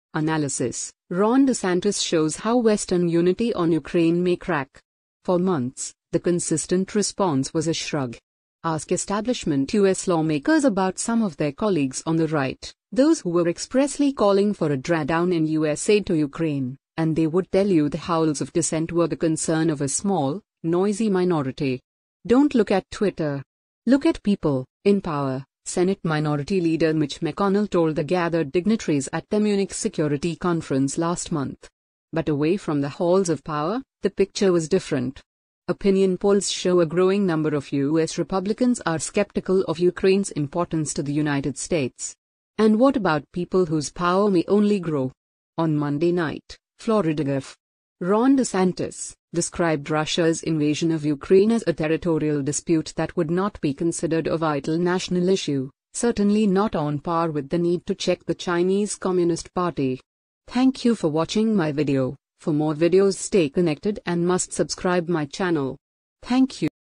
Analysis: Ron DeSantis shows how Western unity on Ukraine may crack
For months, the consistent response was a shrug. Ask establishment U.S. lawmakers about some of their colleagues on the right — those who were expressly calling for a drawdown in U.S. aid to Ukraine — and they would tell you the howls of dissent were the concern of a small, noisy minority.

“Don’t look at Twitter. Look at people in power,” Senate Minority Leader Mitch McConnell told the gathered dignitaries at the Munich Security Conference last month.

But away from the halls of power, the picture was different. Opinion polls show a growing number of U.S. Republicans are skeptical of Ukraine’s importance to the United States.

And what about people whose power may only grow? On Monday night, Florida Gov. Ron DeSantis described Russia’s invasion of Ukraine as a “territorial dispute” that would not be considered a “vital national issue,” certainly not on par with the need to check “the Chinese Communist Party.”